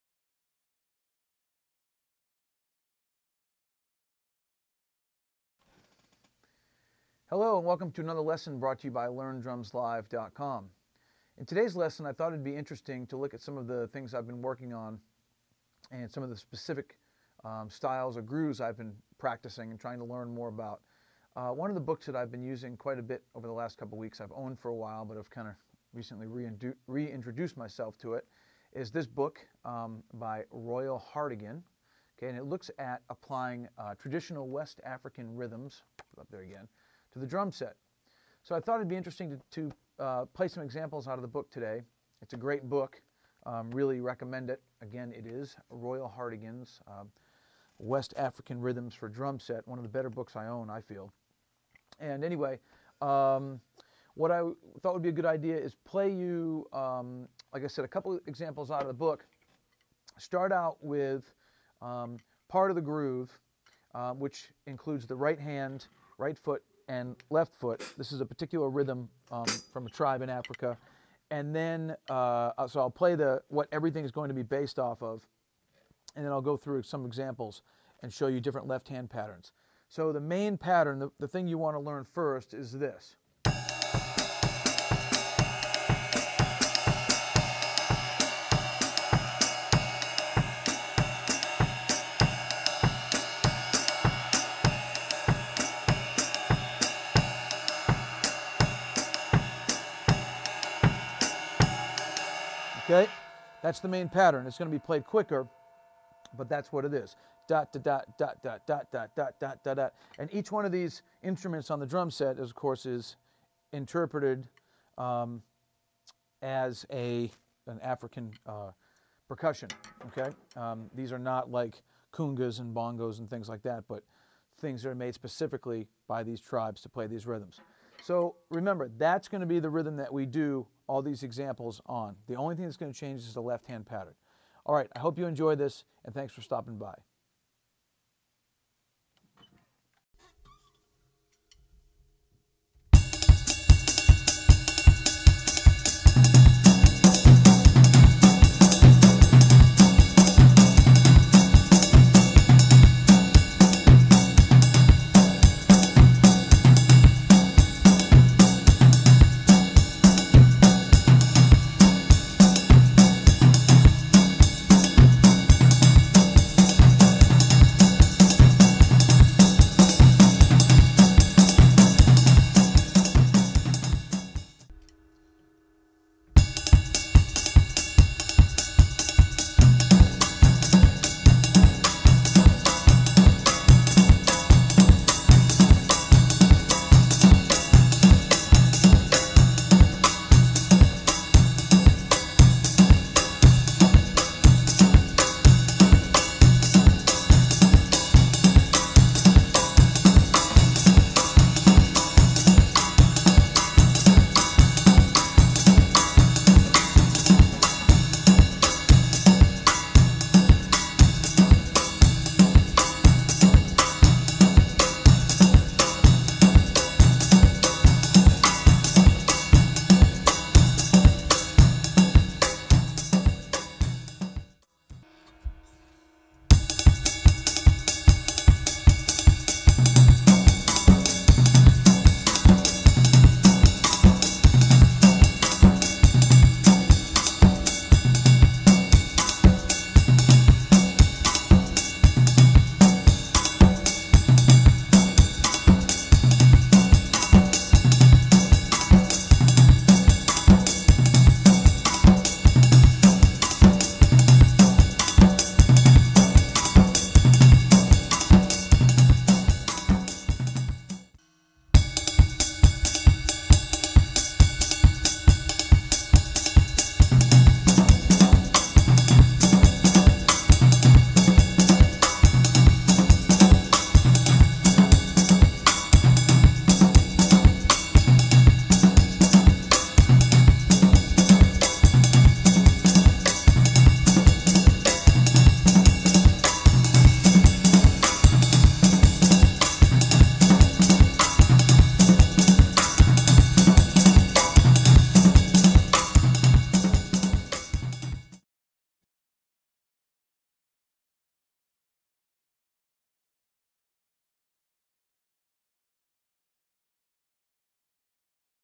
Free Drum Lesson Video: Applying traditional West African Rhythms to the Drumset
These applications are inspired by the writings and research of drummer and educator Royal Hartigan; several variations can be found in his book "West African Rhythms for Drumset." I highly recommend this book. Thank you, Royal, for producing such an informative book. Enjoy!